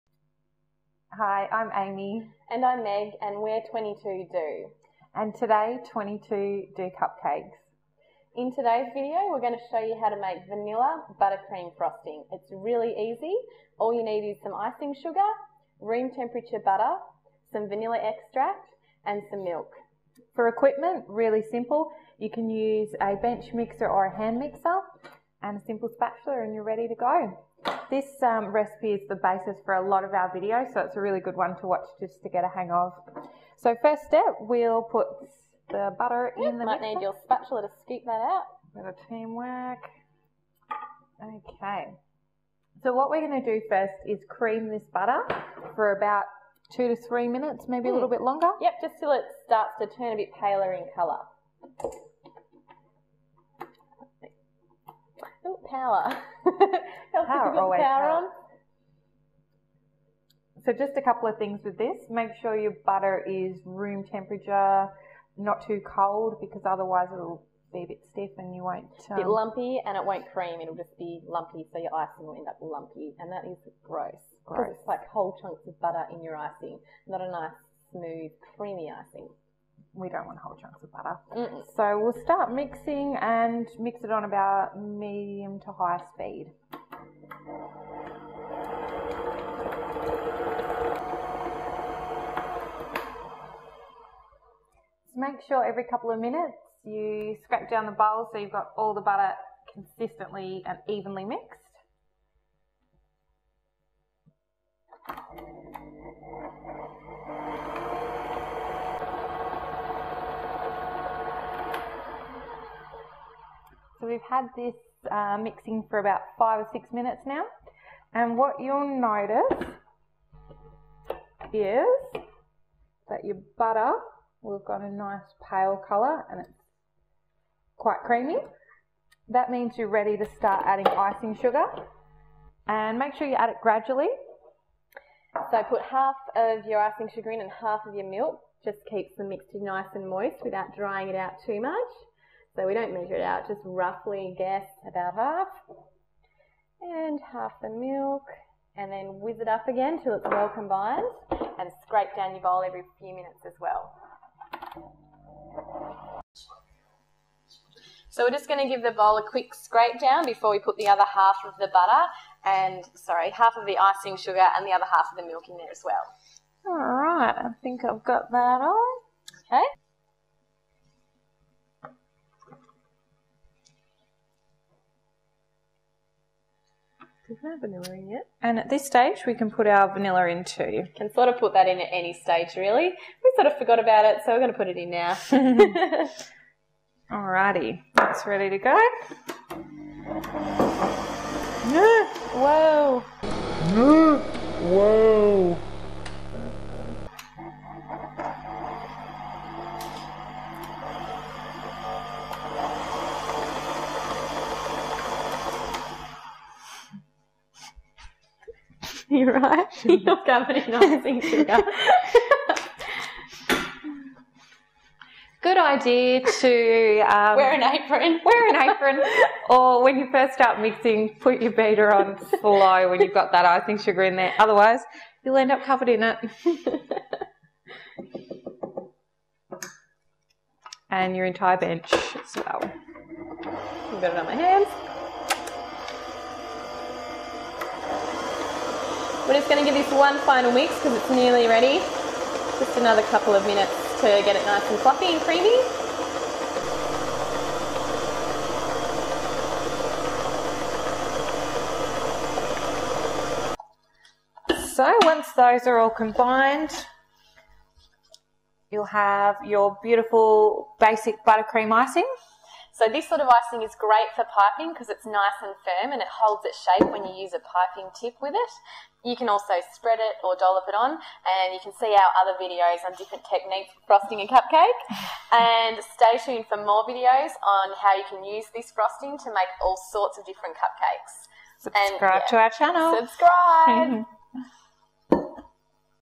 #3 Cupcake Decorating: How to make vanilla butter cream frosting by 22do
Megan and Amy demonstrate how to make basic vanilla frosting
125g butter (room temperature)
1 1/2 cups of icing sugar
2 tablespoons of milk
1 teaspoon of vanilla extract